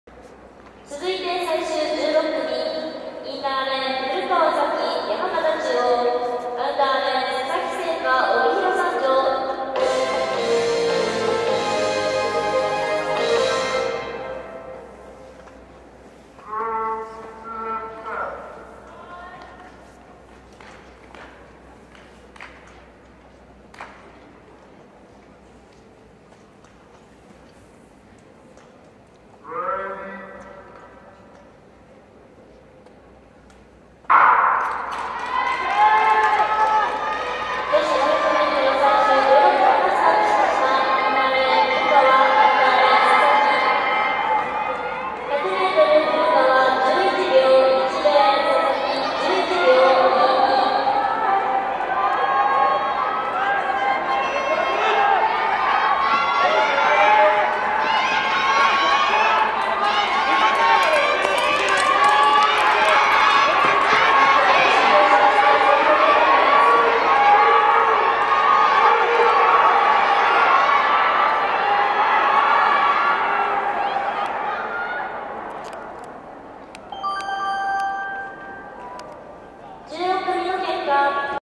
2019年2月24日 高校選抜 女子500m 16組 out
2019年2月24日 高校選抜 女子500m 16組 out
16  IN  古 川 幸 希  山形中央高  00:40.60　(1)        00:11.10 [00:11.10]  00:40.60 [00:29.50]
OUT  佐々木 成 果  帯広三条高校  00:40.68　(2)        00:11.22 [00:11.22]  00:40.68 [00:29.46]

平成30年度 第9回全国高等学校選抜スピードスケート競技会
2019年2月23-24日
会場